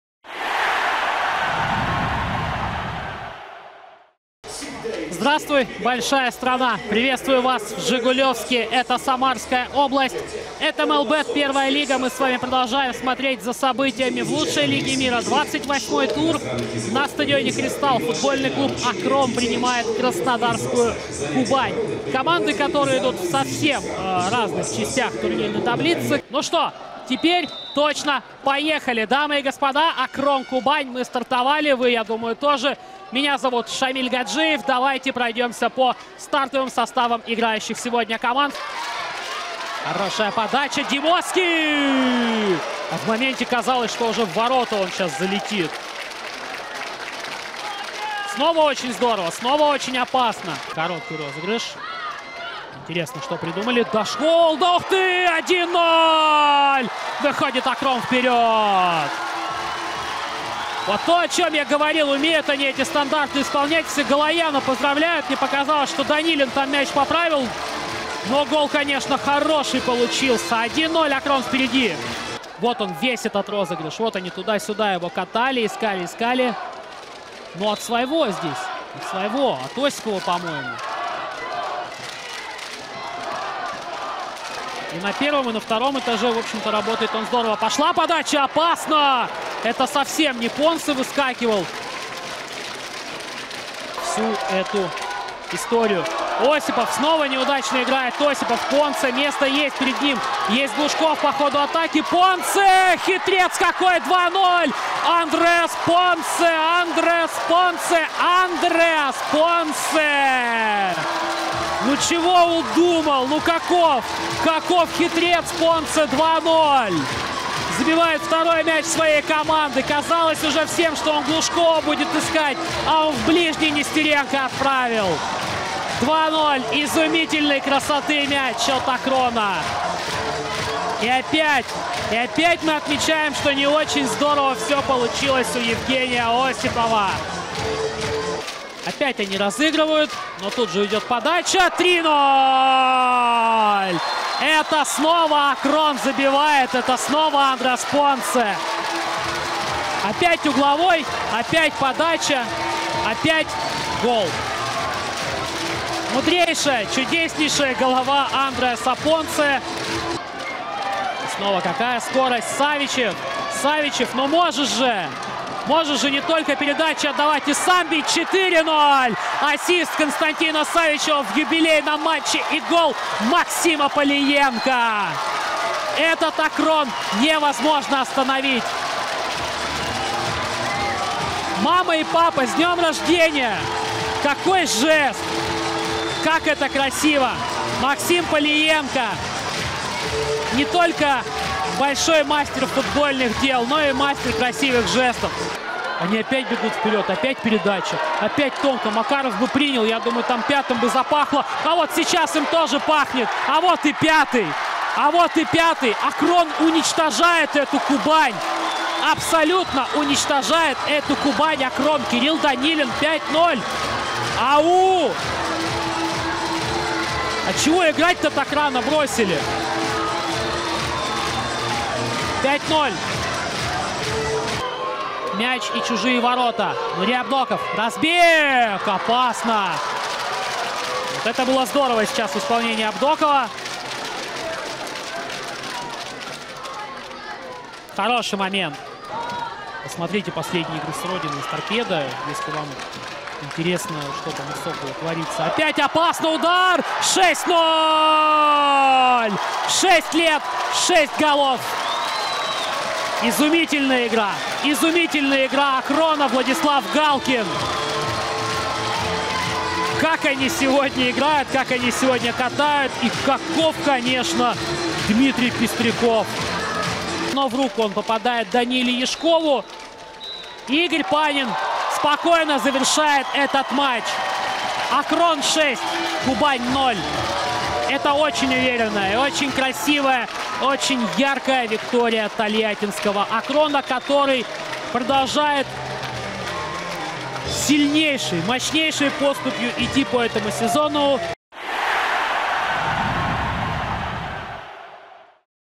Обзор матча «Акрон» — «Кубань» | 28 тур МЕЛБЕТ-Первой Лиги
Лучшие моменты из игры «Акрон» (Тольятти) и «Кубань» (Краснодар) 20 апреля 2024.